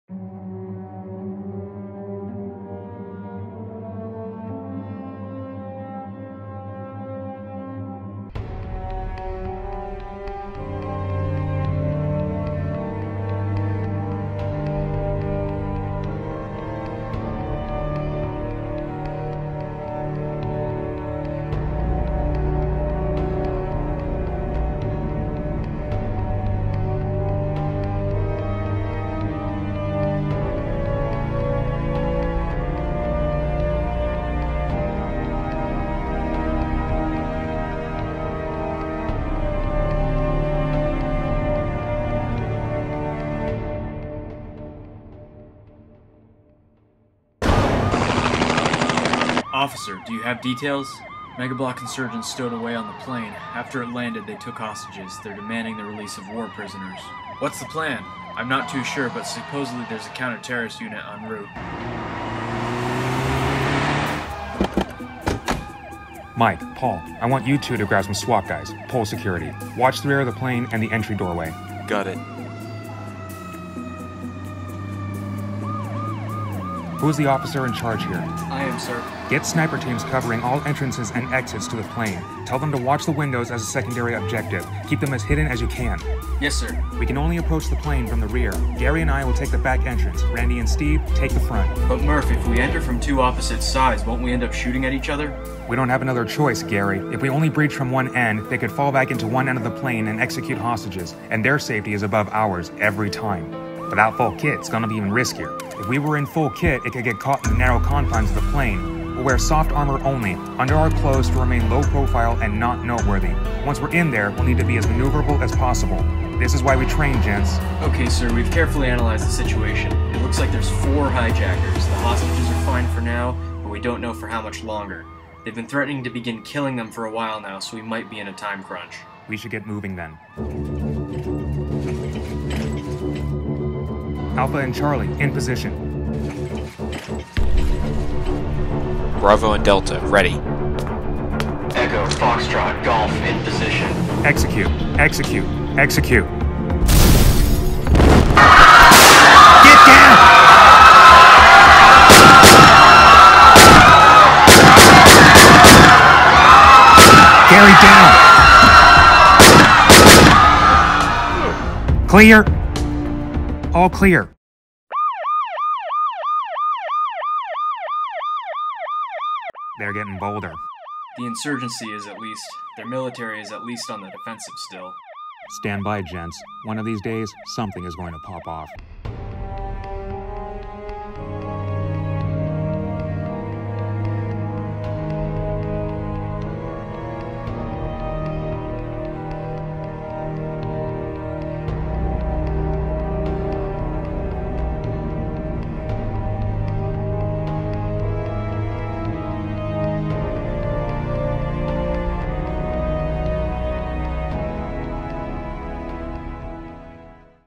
Lego Vs. Mega Bloks Season 2 Episode 1: Hijacked
A continuation of Series 3, been working with a new camera so the image margins are weird, it has since been fixed but this episode has the annoying black bars on the side.